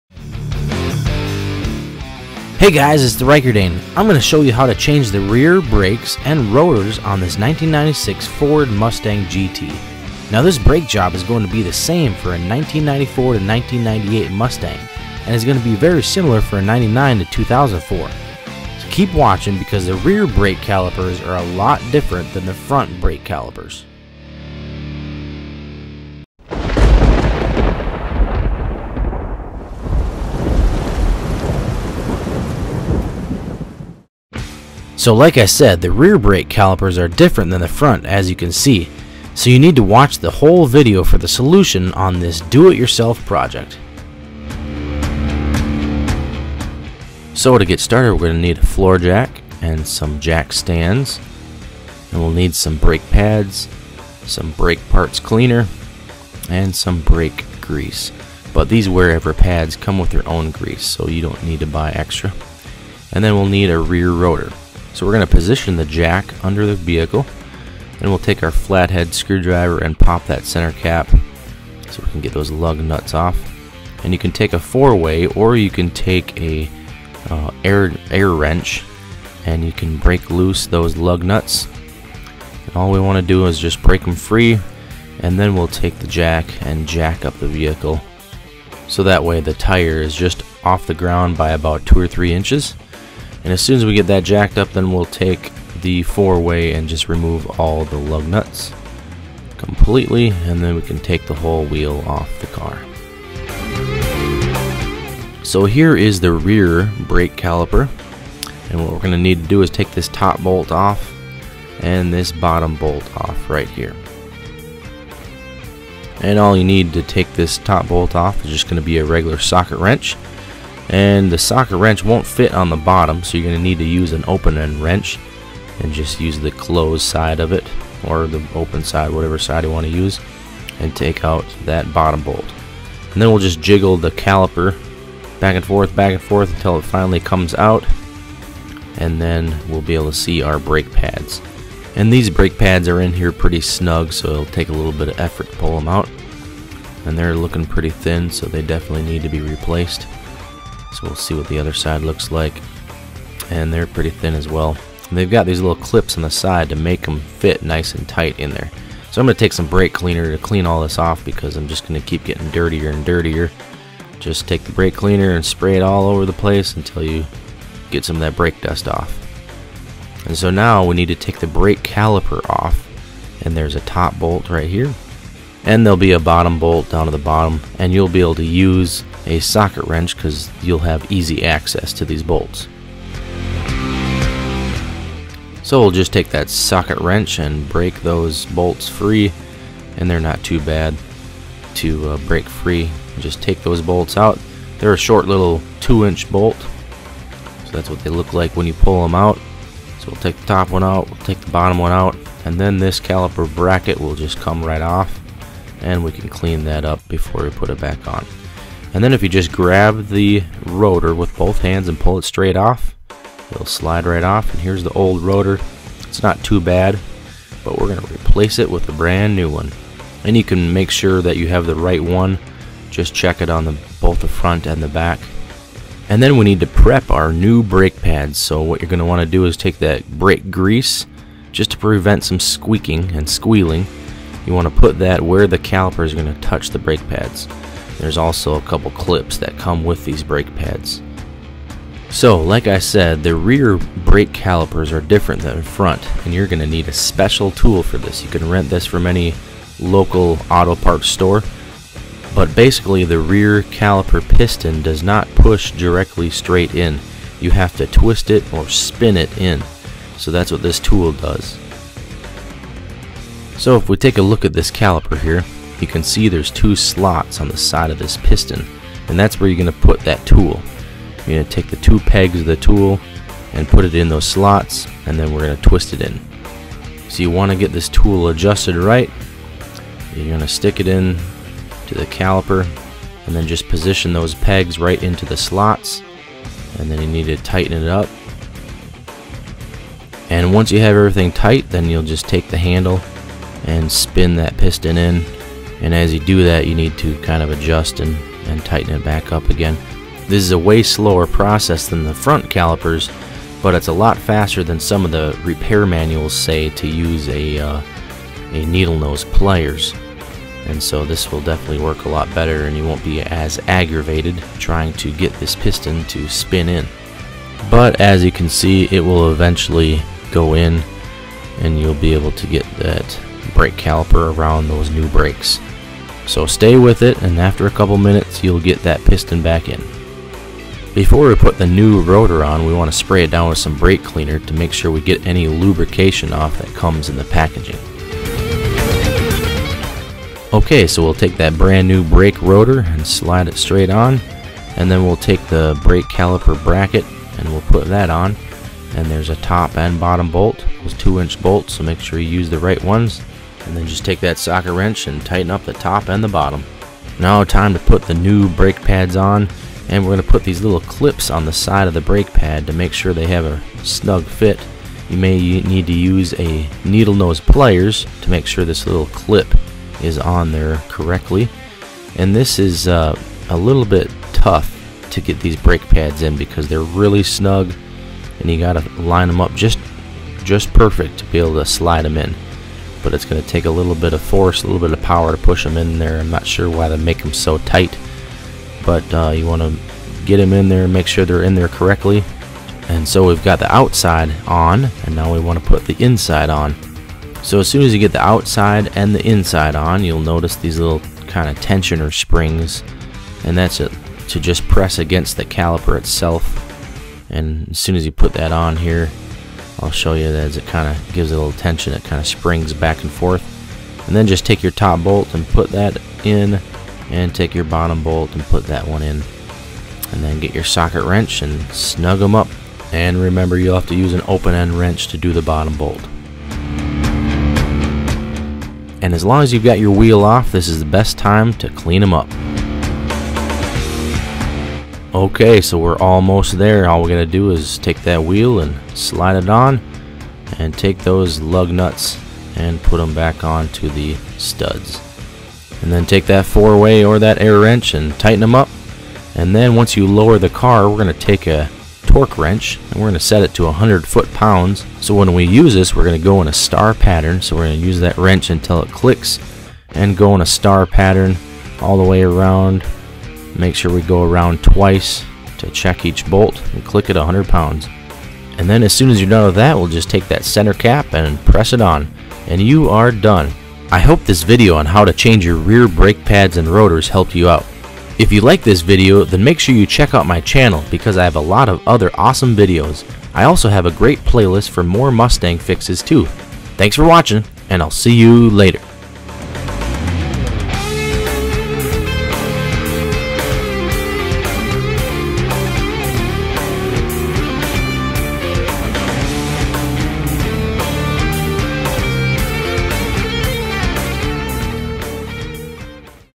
MUSTANG REAR BRAKES AND ROTORS GT 4.6 5.0 1994-2004
Watch how to replace and install your rear brake pads and rotors on your 1994 to 2004 Ford Mustang. The rear brake calipers are different than the front so you will need a special tool that you can get from  your local auto parts store. Watch to see how to do it yourself and save some money at the same time!

If you like seeing interesting videos, workout & exercise videos, food & nutrition, how to vids, racing, autos, and do it yourself projects then...